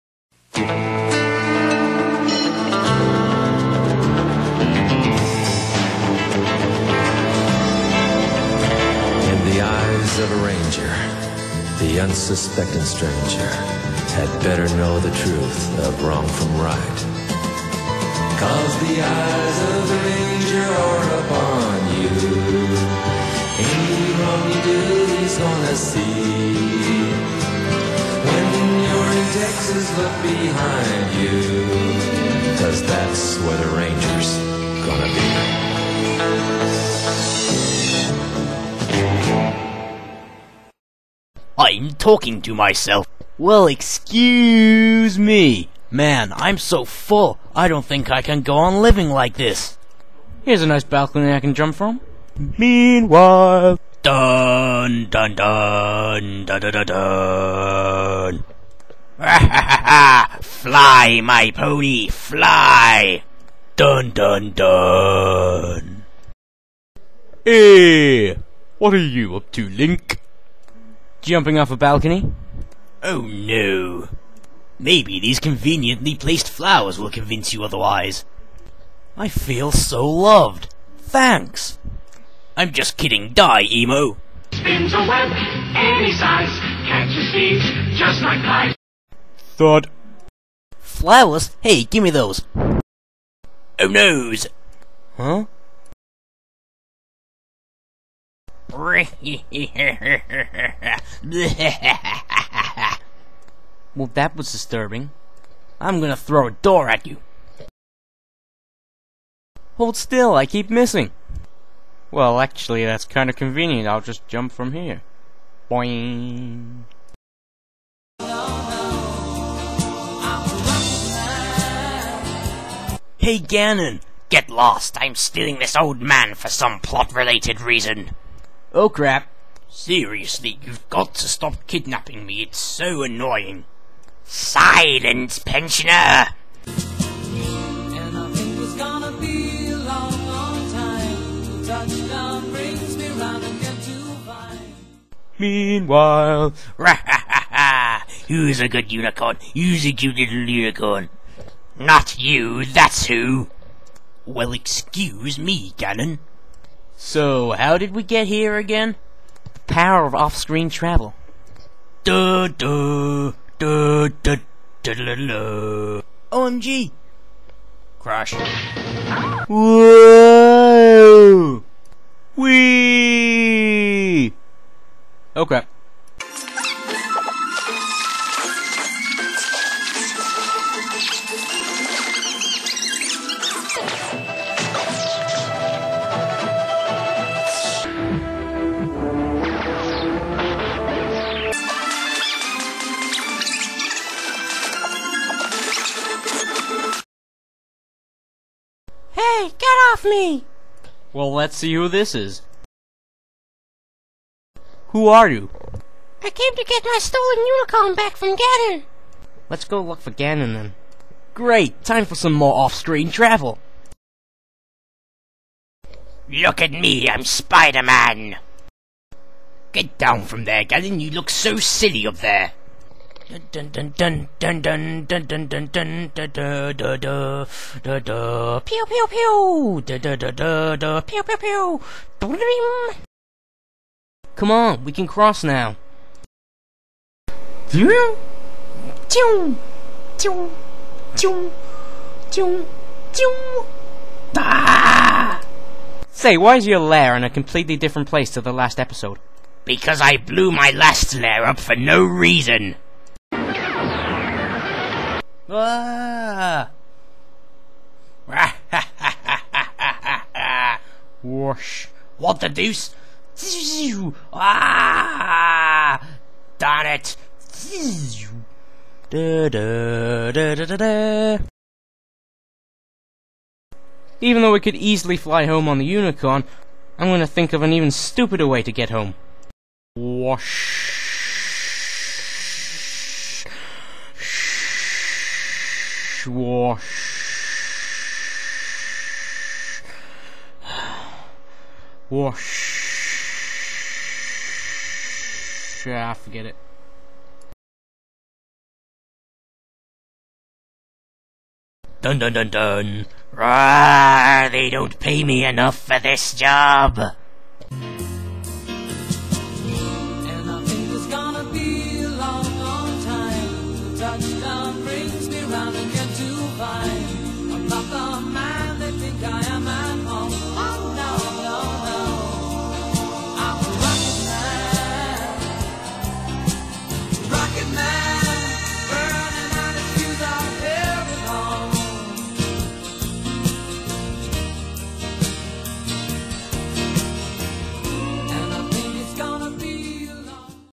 Zelda the Abridged series - Episode Two
Ganon steals a winged horse